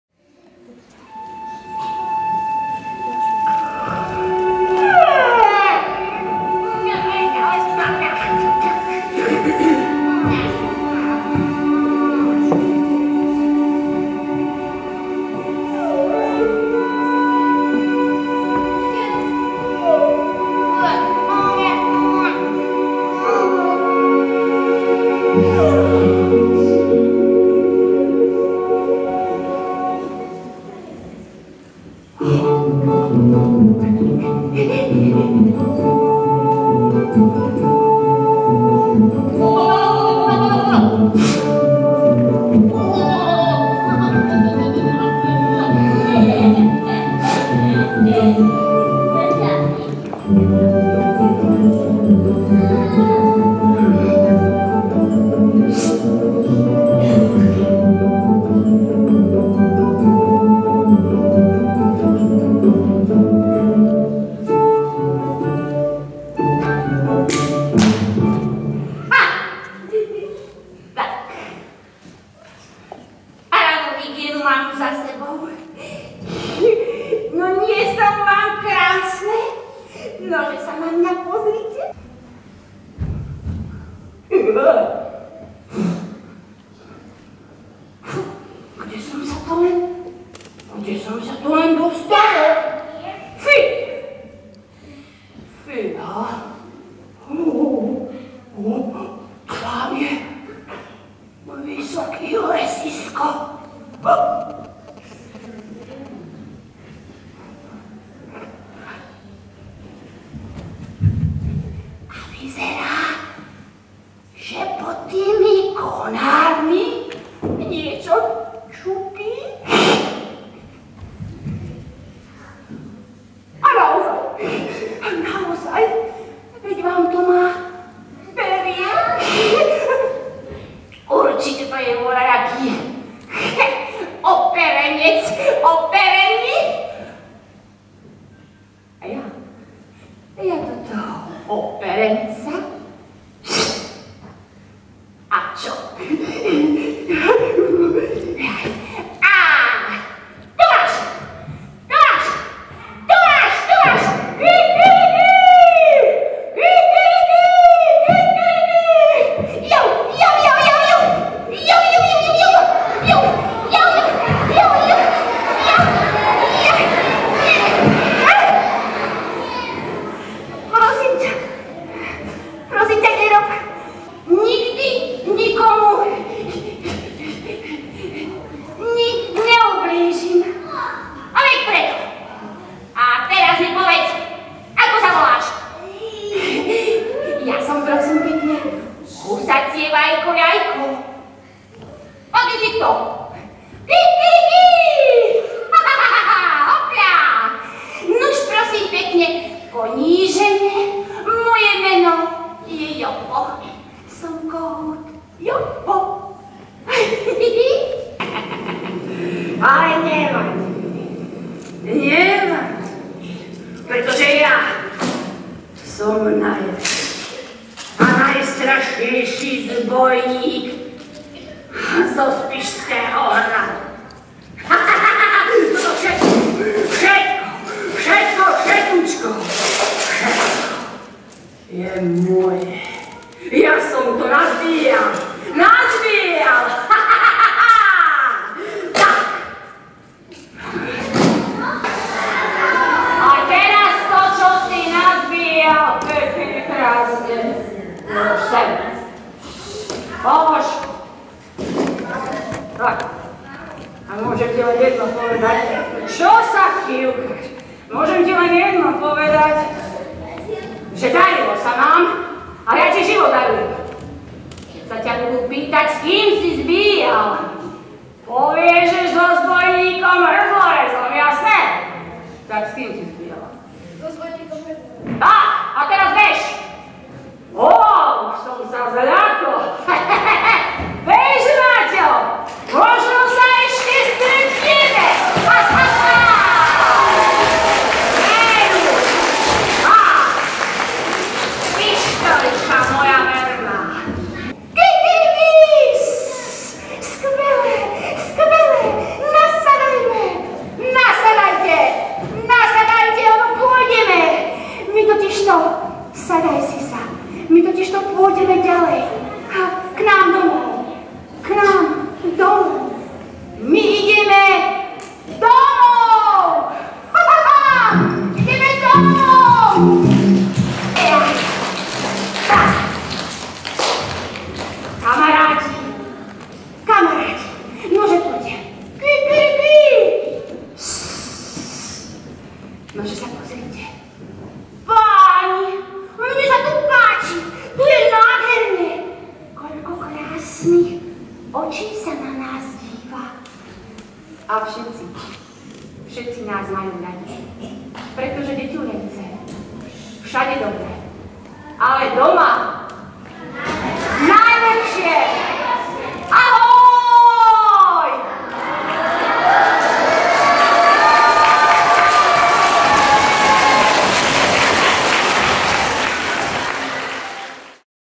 46. Palárikova Raková: Ako šlo vajce na vandrovku
Ukážka z pohostinného divadelného predstavenia divadla Ofina z Ružomberku v rámci 46. ročníka Palárikovej Rakovej v Starej Bystrici :-D.